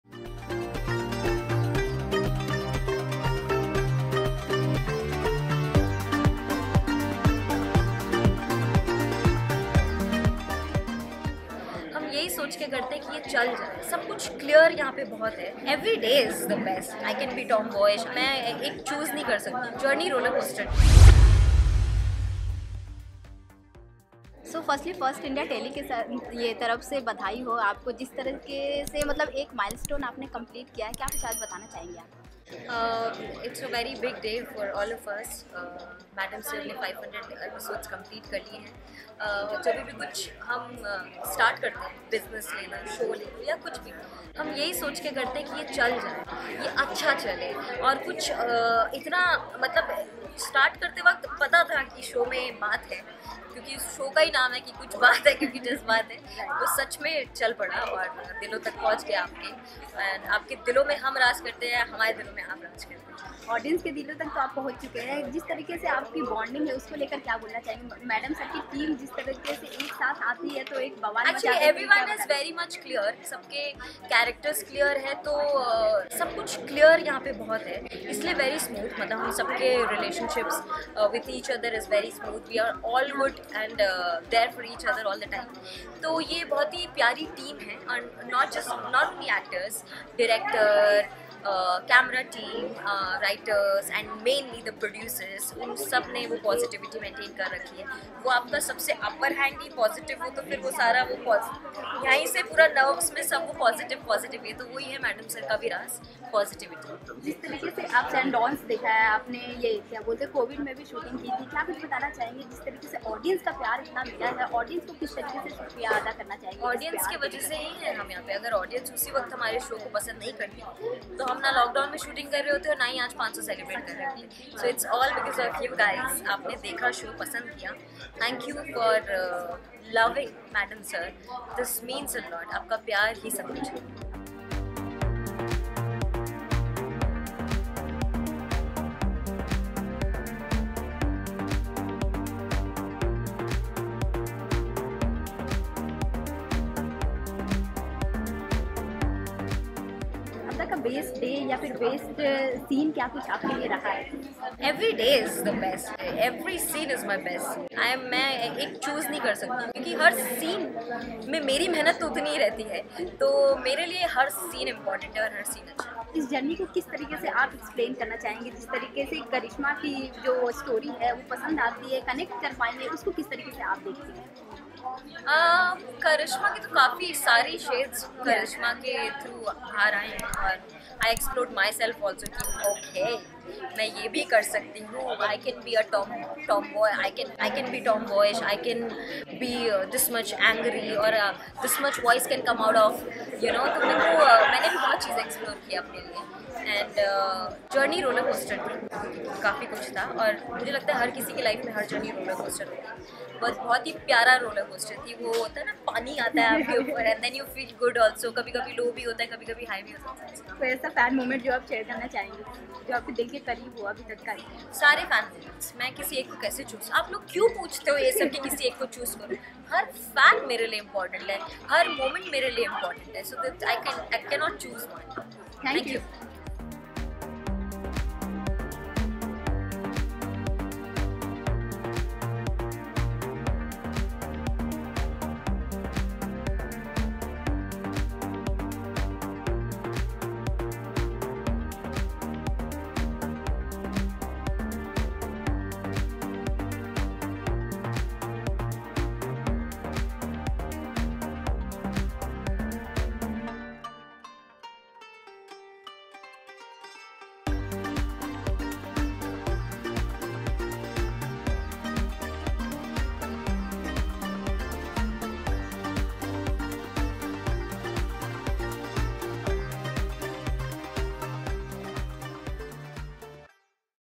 Maddam Sir के 500 Episode Complete होने पर Yukti Kapoor ने First India Telly संग बाटी अपनी ख़ुशी
Maddam Sir: कुछ बात है क्योंकि जज्बात है एक कॉमेडी show- Action Television Series यह शो चार महिला पुलिस अधिकारियों के इर्द-गिर्द घूमता है, जो यहां काम करती हैं एक महिला पुलिस स्टेशन जिसमें एसएचओ हसीना मलिक है, करिश्मा सिंह, हेड कांस्टेबल पुष्पा सिंह, कांस्टेबल संतोष शर्मा, और कांस्टेबल चीता चतुर्वेदी। वे अक्सर रोमांच से भरे नए नए Cases सुलझाते हुए नज़र आते है। वही इस Show Maddam Sir में है आज का दिन काफी ख़ास First India Telly आपके लिए Show के Set से लाया है कुछ ख़ास झलकियां जहा है माहौल जश्न का। क्यूंकि Maddam Sir ने पूरे कर लिए अपने 500 Episodes जिसकी खुसी Show के सभी Cast and Crew Members ने मिलकर मनायी।  मज़े की बात ये रही इसमें की की Show के कलाकारों के साथ साथ  Show की Team ने आज नज़र आयी Police Uniform में। इस ख़ास मौके पर Show की Actress Yukti Kapoor AKA Karishma Singh ने First India Telly की Exclusively बातचीत और Share की अपनी ख़ुशी और बतया और क्या कुछ होनेवाला है खास इस show में। जश्न की Exclusive Video देखें सिर्फ First India Telly पर।

Reporter: Prity Rani
Cameraman: Krishna Mishra
Editor: Abhishek Singh
Producer: Sonia Jaiswal